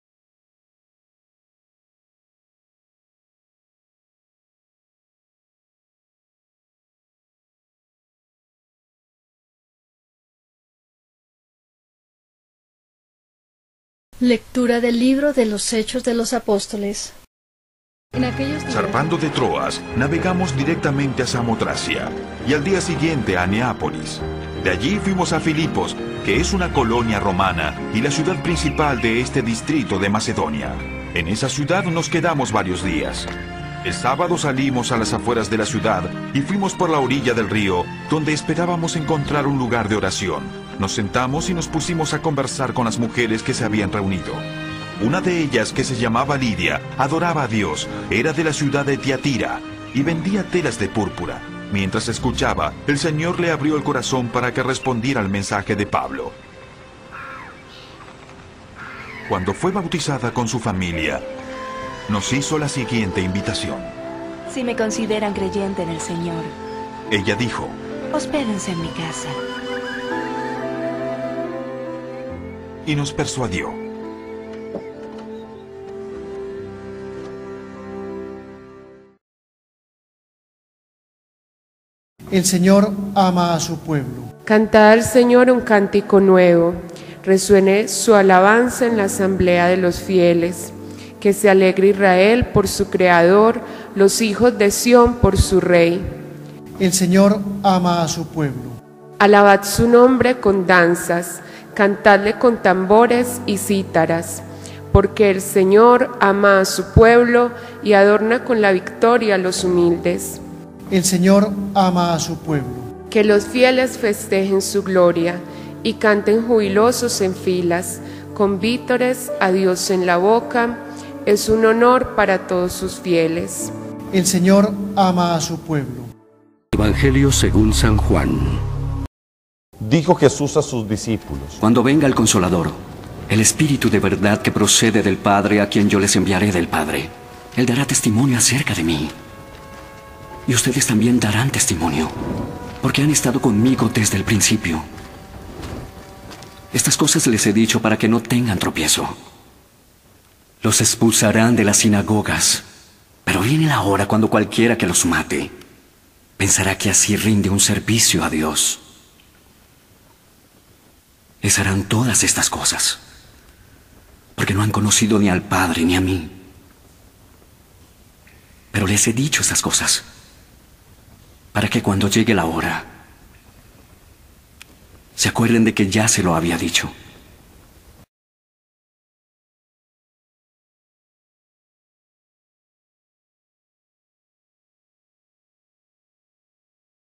Evangelio Lunes 27 de mayo de 2019 VI SEMANA DE PASCUA | JUAN 15,26–16,4a
Evangelio y lecturas del LUNES VI SEMANA DE PASCUA DE RESURRECCIÓN MAYO 27 DE 2019 Primera Lectura: Del libro de los Hechos de los Apóstoles 16,11-15 Salmo 149, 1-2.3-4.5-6a.9b R/. El Señor ama a su pueblo Del Santo Evangelio según San JUAN 15,26–16,4a